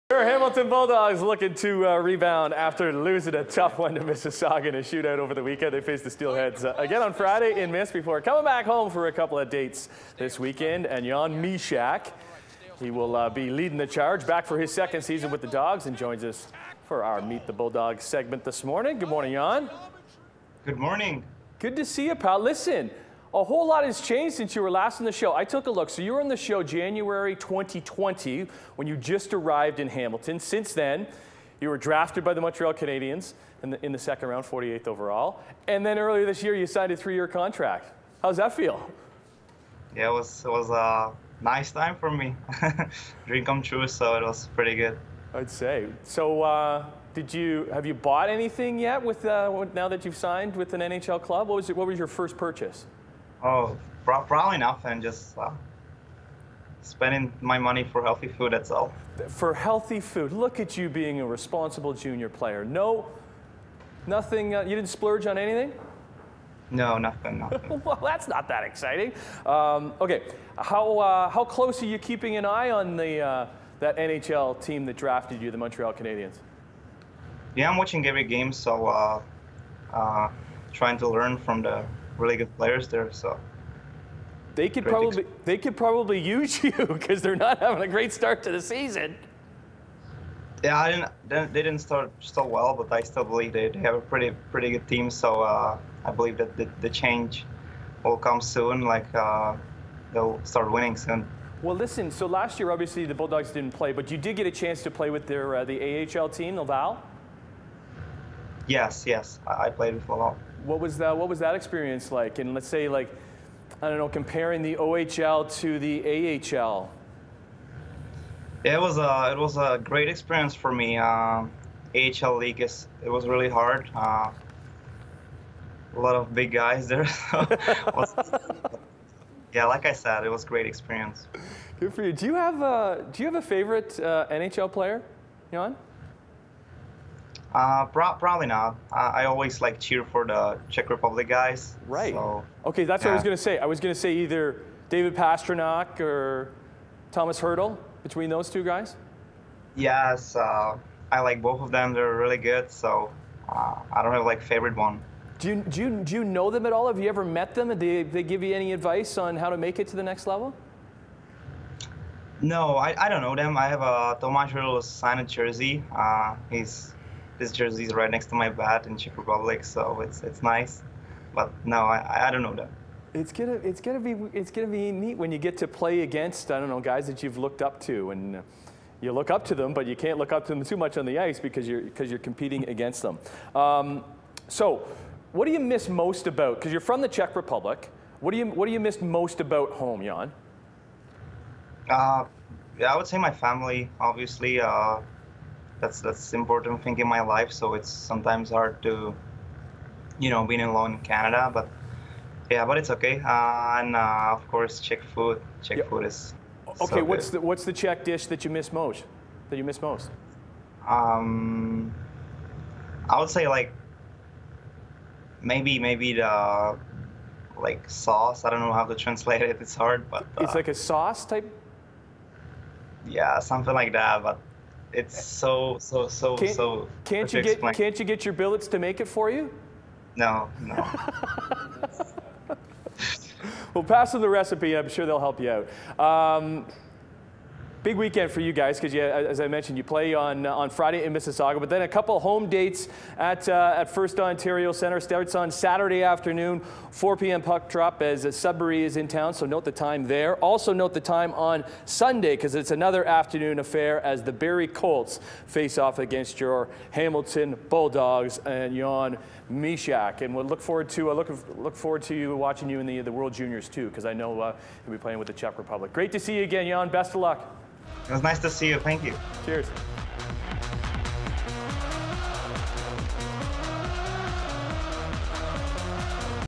Hamilton Bulldogs forward shares experience playing in AHL last season
The Hamilton Bulldogs are looking to rebound after losing to Mississauga in a shootout over the weekend.  This Friday they will face the Steelheads again in Mississauga before playing at home Saturday and Sunday.  Jan Mysak will be leading the charge, as he is back for his second season with the Bulldogs and joined us for our Meet the Bulldogs segment.